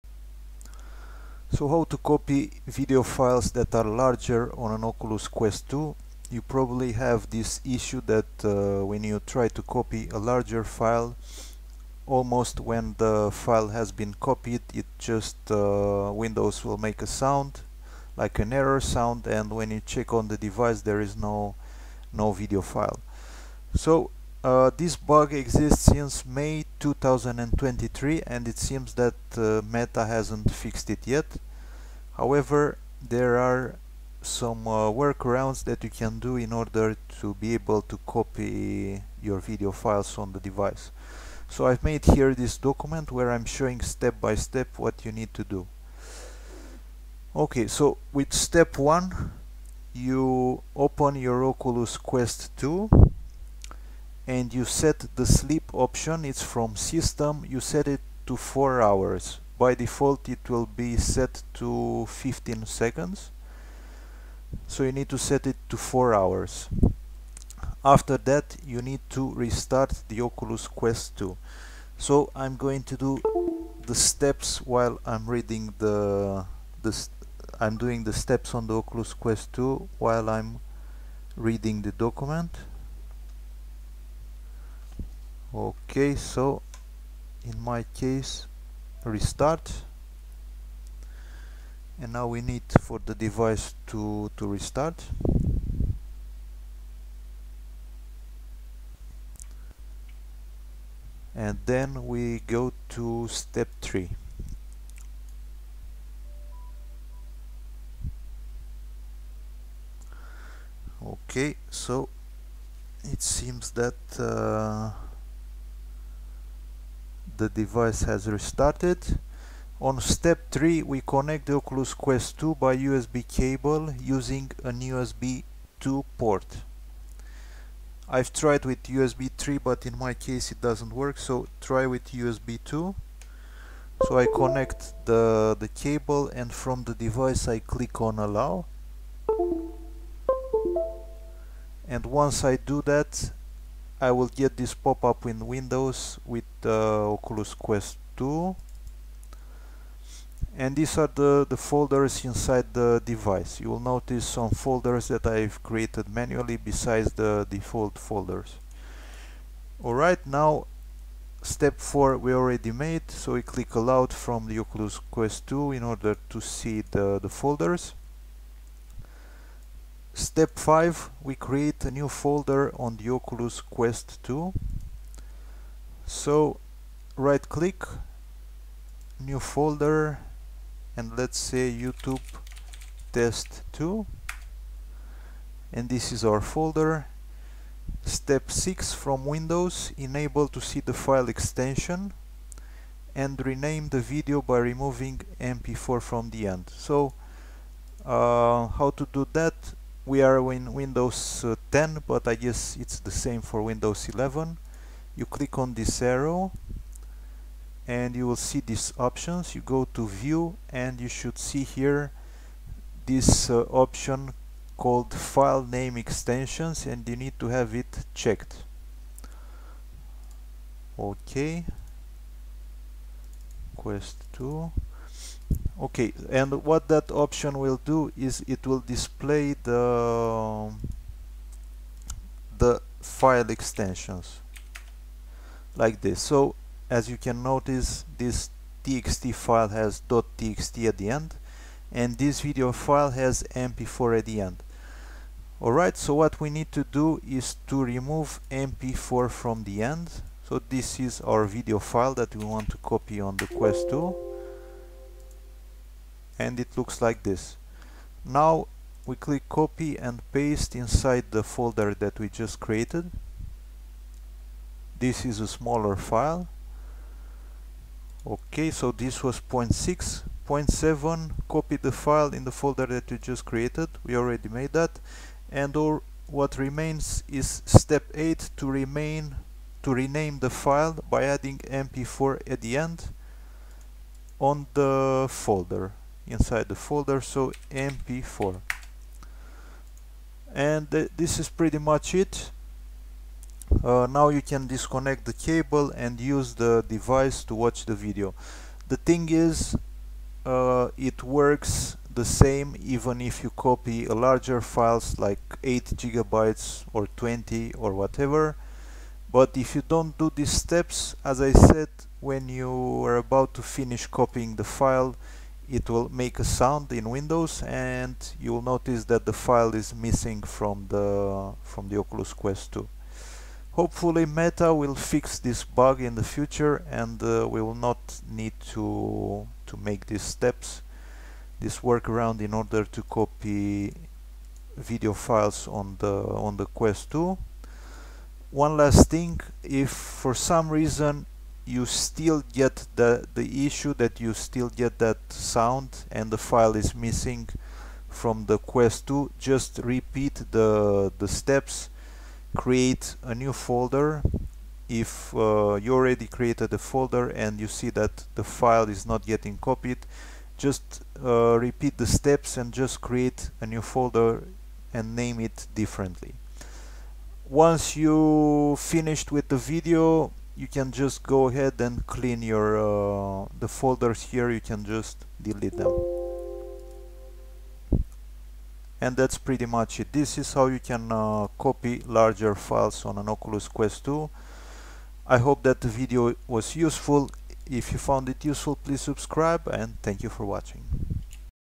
Cannot copy larger video files on an Oculus Quest 2 Fix
Follow the steps:

1. Open the Oculus Quest 2 and set the “Sleep” option after 4 hours

2. Restart the Oculus Quest 2

3. Connect the Oculus Quest 2 by USB cable, use a USB 2 port

4. Click “Allow” from the Oculus Quest 2, the window that will appear in the device

5. Create a new folder on the Oculus Quest 2

6. From Windows enable to see the file extension and rename the video file by removing “.mp4” from the end

7. Copy the file as it is on the new folder that you just created.

8. Rename the file by adding “.mp4” at the end.